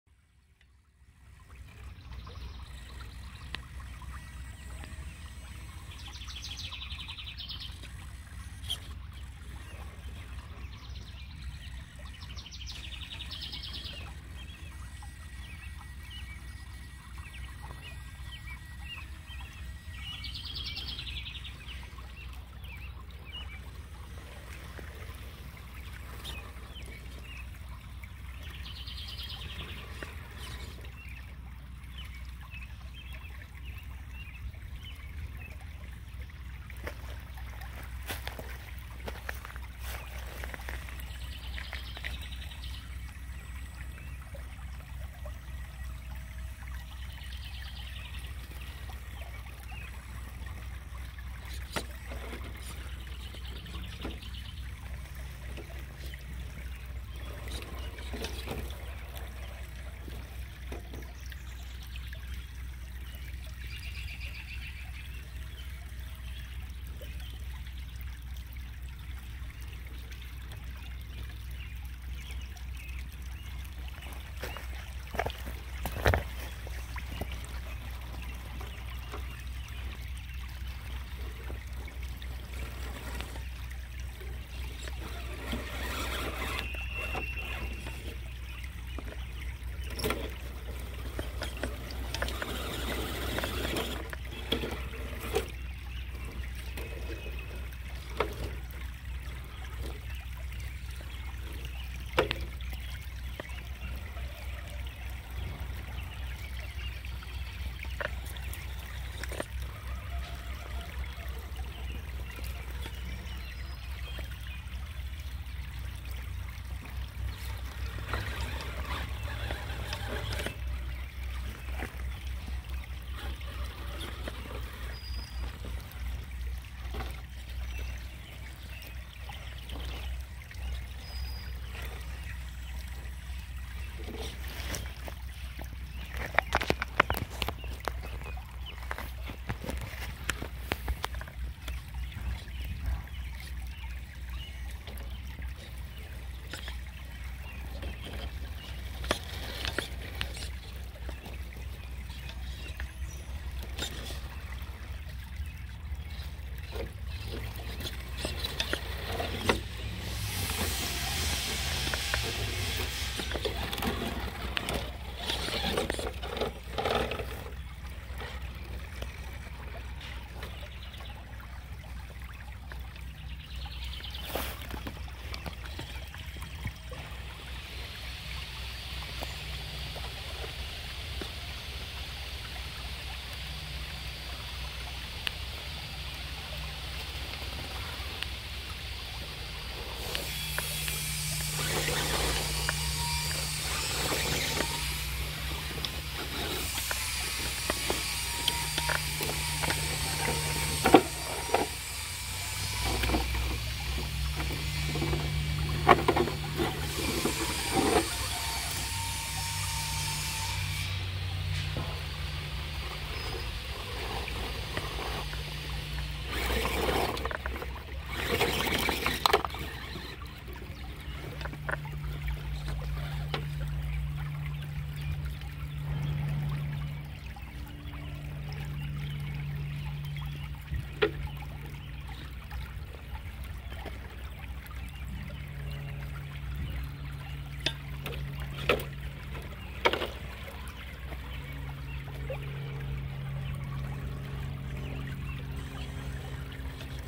New build with rc4wd weighted rim's pro-line tires 1800kv hobbywing axe and 7.4 savox steering servo
New build